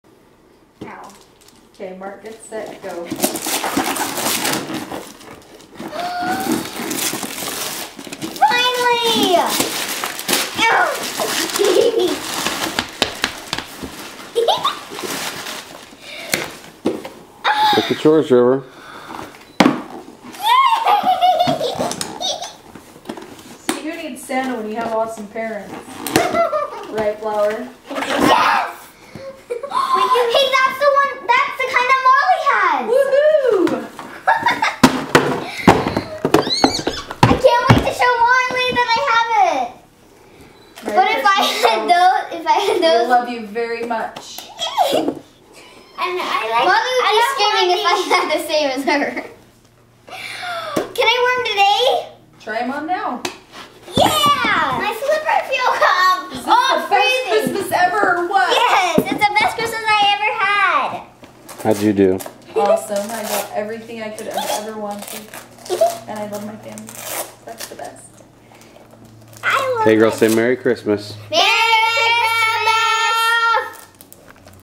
Sketchers Twinkle Toes
The girls opening their most prized Christmas gift of 2013.